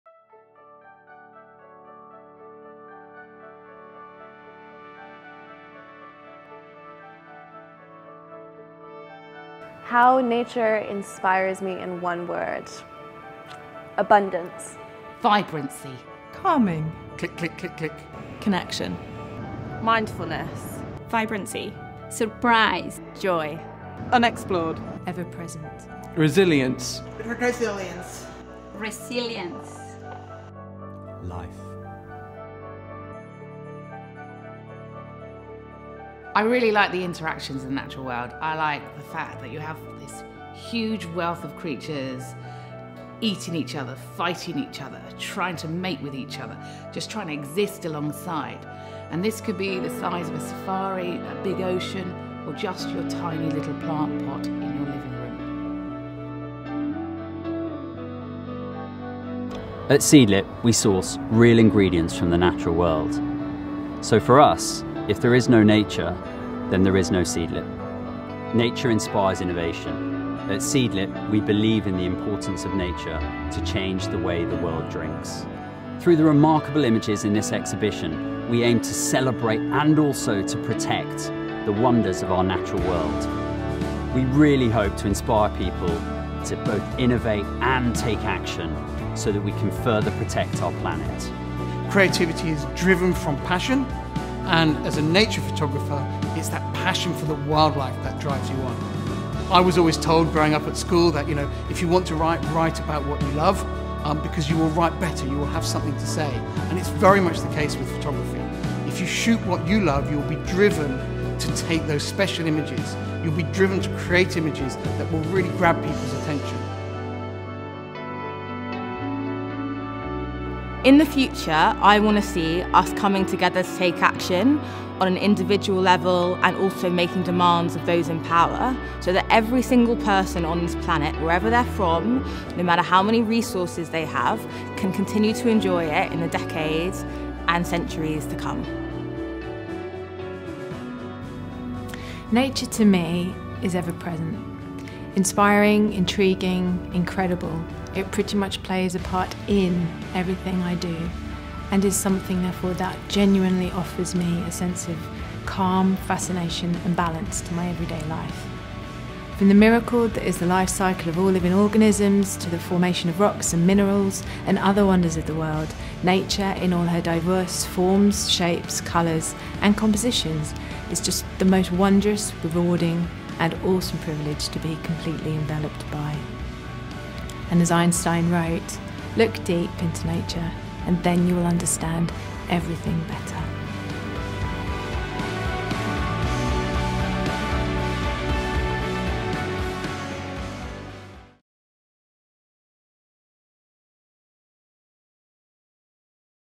Inspiration from Nature | Wildlife Photographer of the Year | Natural History Museum
How does nature inspire you? Wildlife Photographer of the Year brings all the beauty and wonder of the natural world to you. The time is here to act for nature.